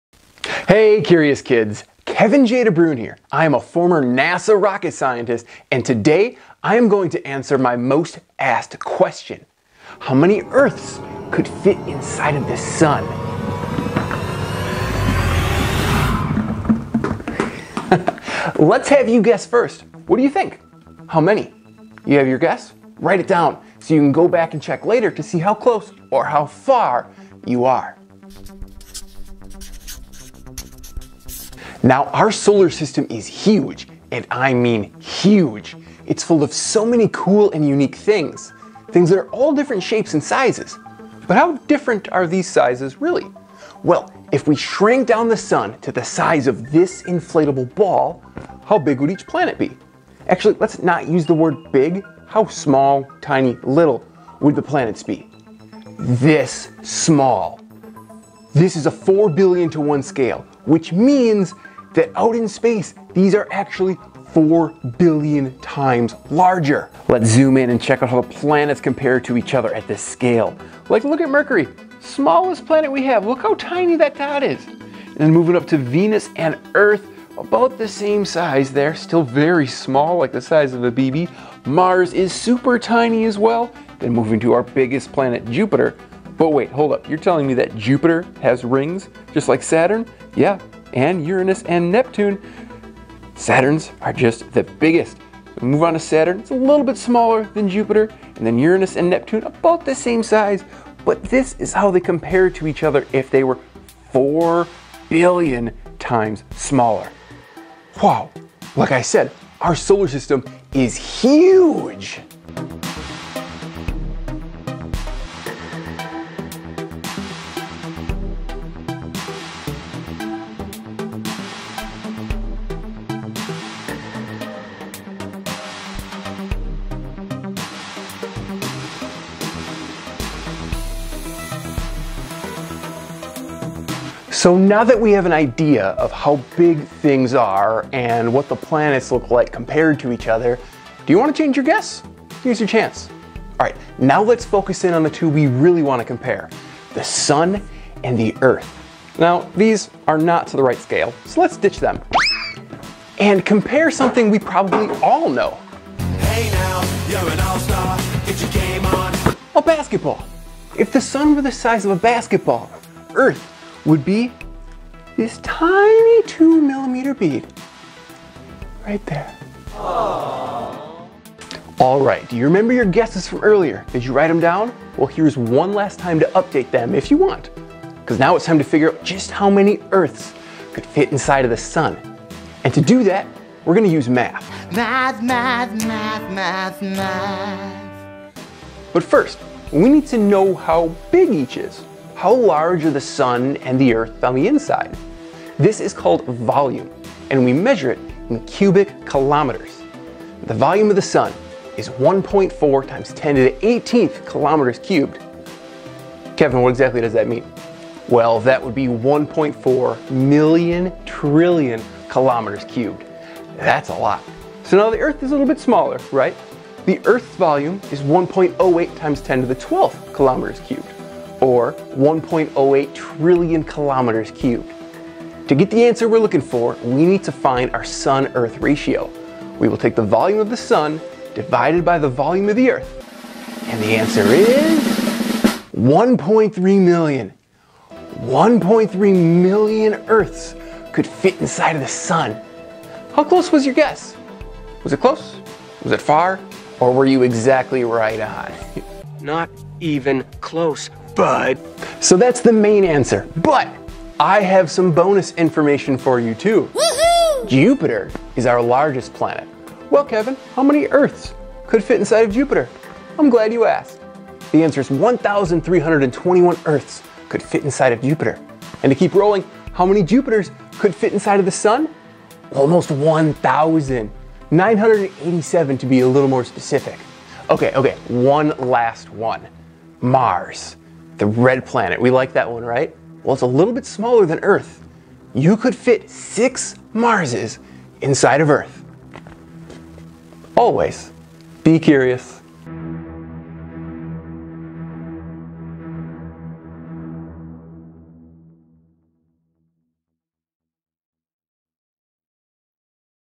How Many Earths Could Fit Inside the Sun?
I revisit my most asked question! Do you have a guess? Put it in the comments and see if you're right!